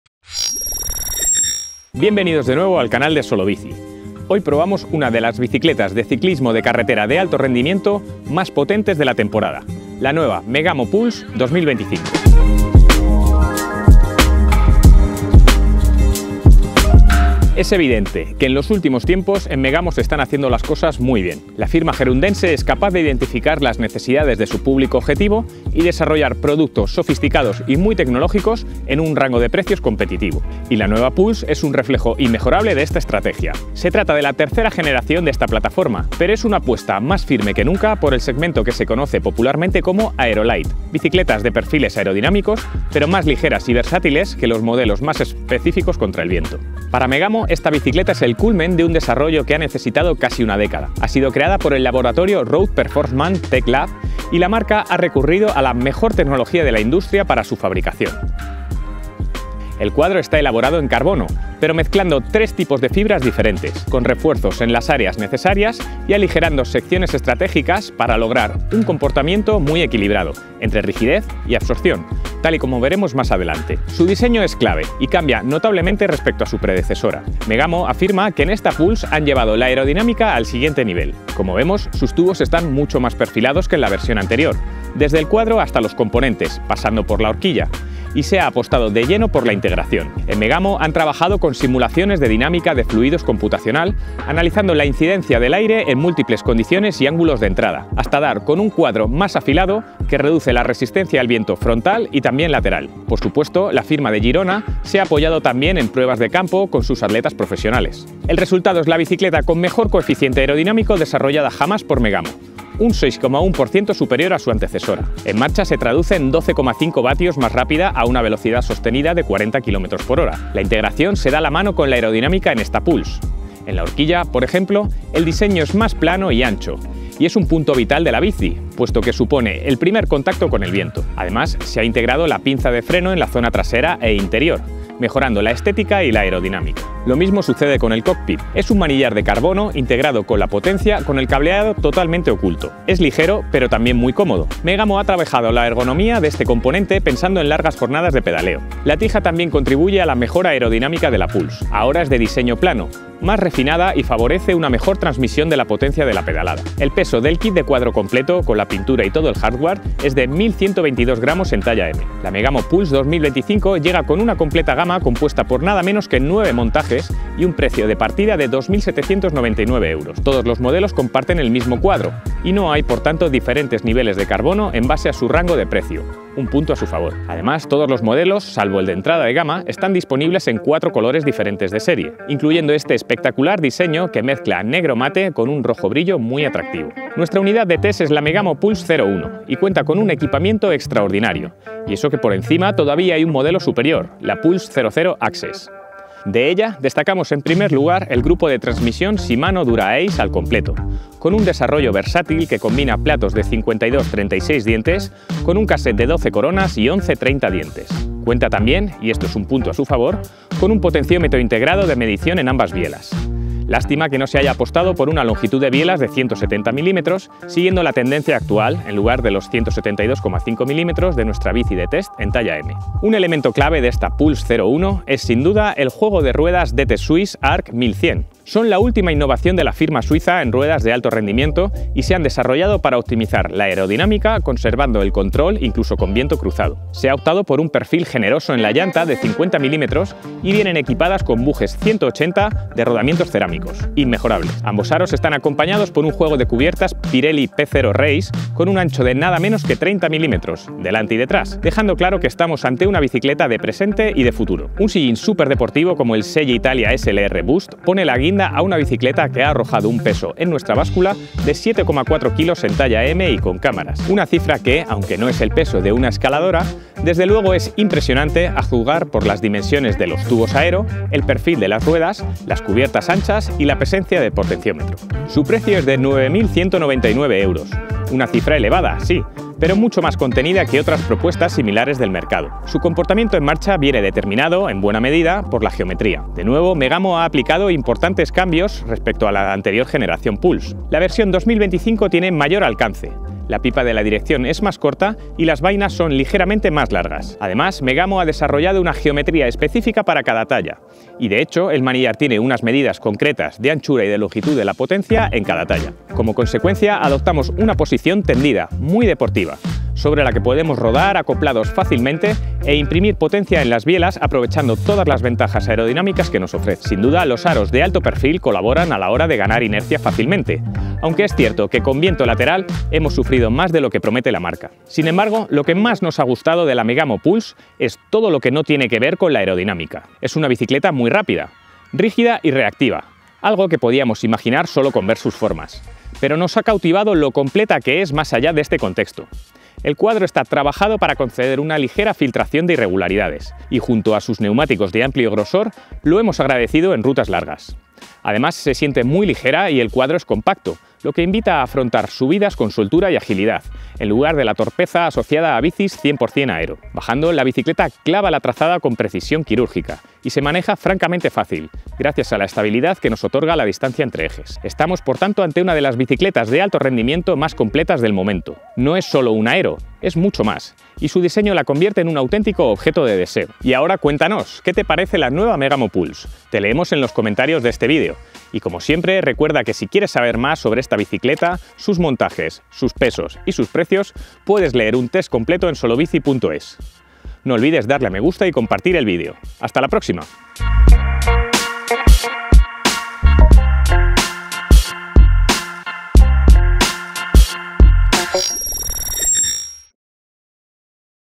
Megamo Pulse 2025. La bicicleta de carretera total: aero, ligera y cómoda
La nueva Megamo Pulse apuesta fuerte en su tercera generación, tratando de convertirse en la bicicleta total, con un cuadro más aerodinámico, más ligero y más cómodo. La hemos probado a fondo para descubrir cómo es la bicicleta más rápida desarrollada por la marca gerundense.

para estar al día de toda la actualidad del ciclismo.